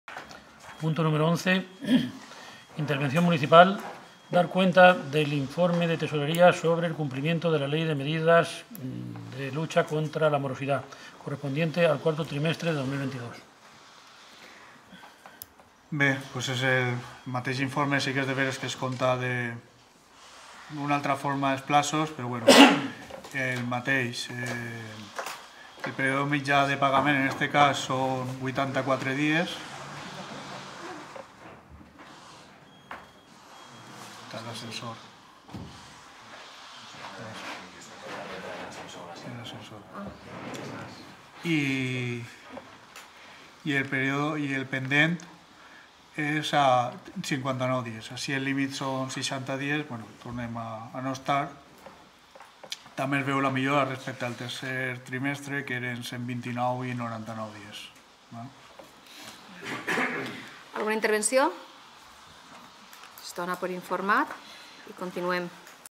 COMPLIMENT LLEI CONTRA MOROSITAT 4T 2022 - Ple Xixona Febrer 2023
11. Dació de compte de l'informe de tresoreria sobre el compliment de la Llei de mesures de lluita contra la morositat, corresponent al quart trimestre de 2022.

Punt corresponent a la sessió ordinària del mes de febrer del Ple de la Corporació que va tindre lloc el 23 de febrer de 2023.